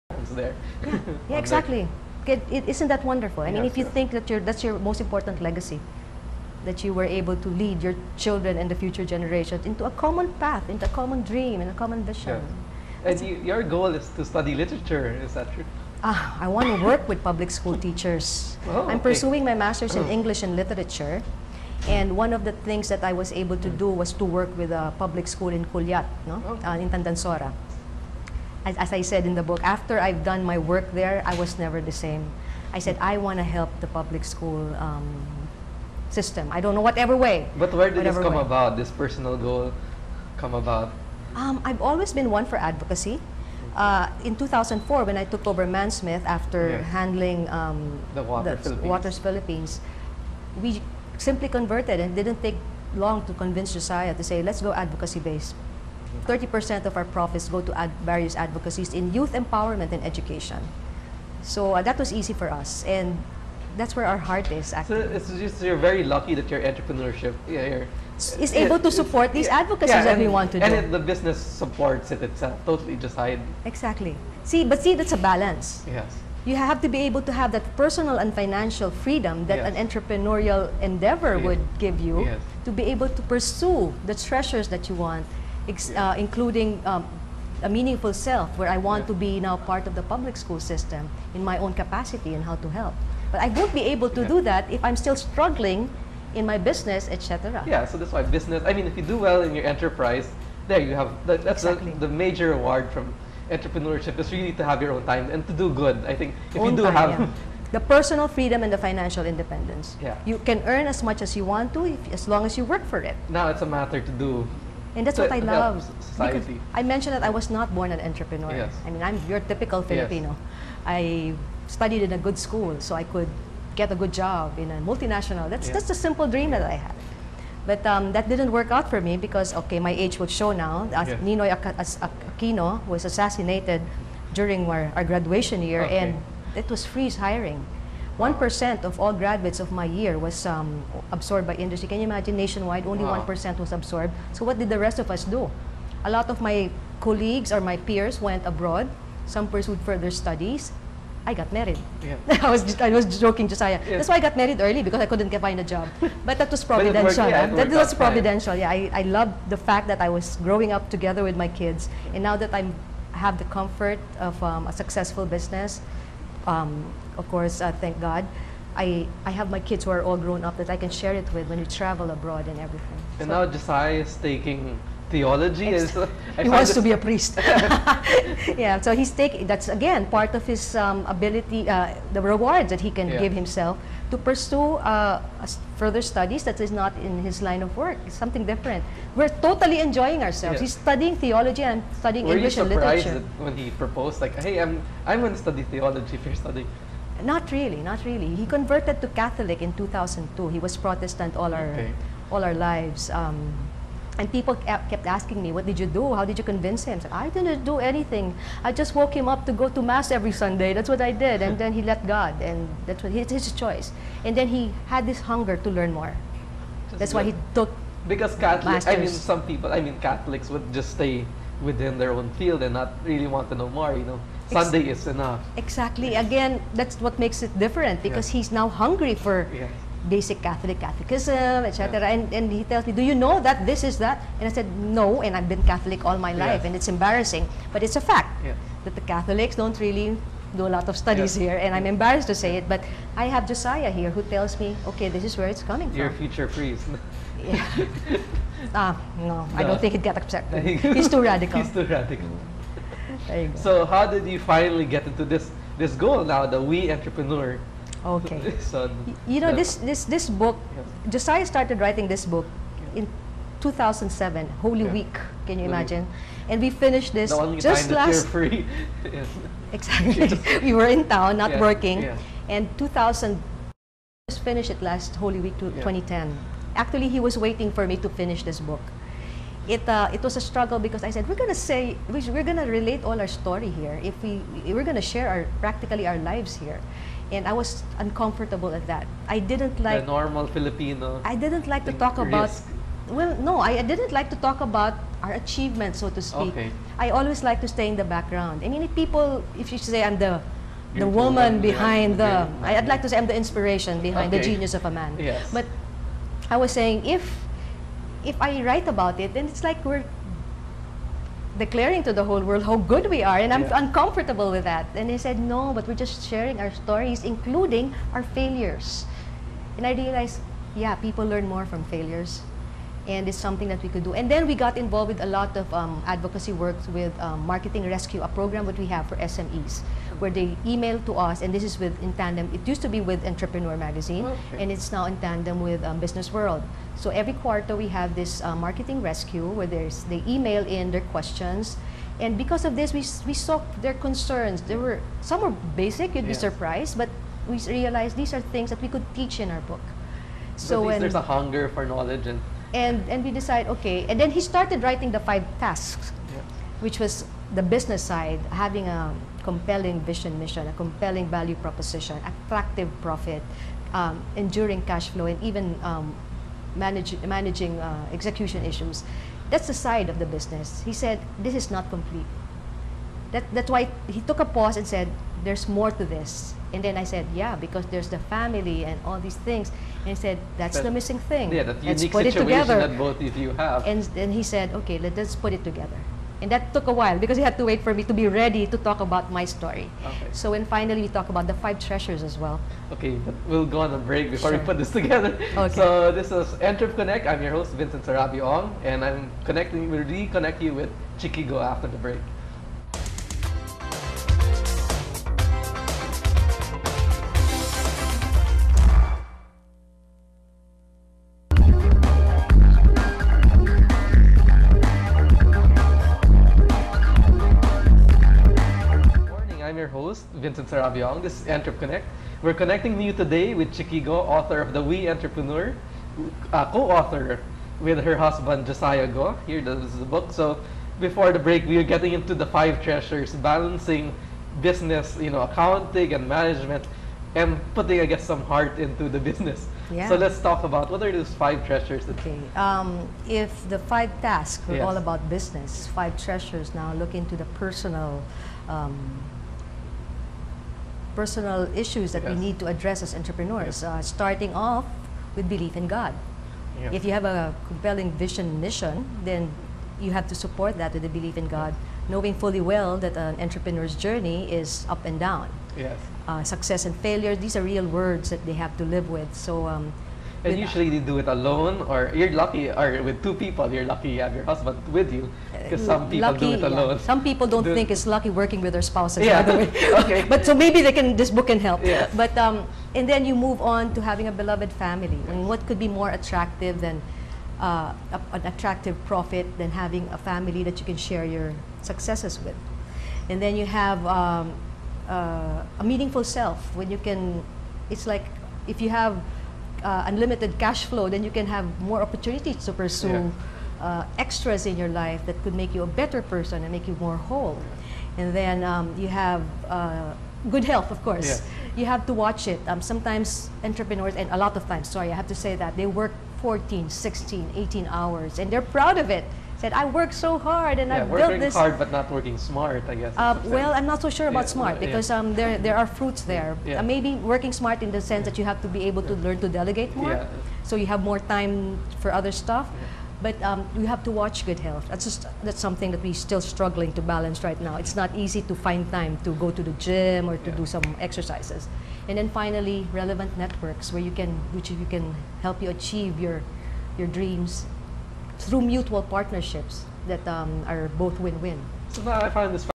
EntrepConnect: The WE Entrepreneur Chiqui Escareal-Go Part 3
Chiqui Escareal-Go, president and CEO of Mansmith and Fielders, talks about her and husband Josiah Go's new book The WE Entrepreneur. She discusses the five treasures of every entrepreneur.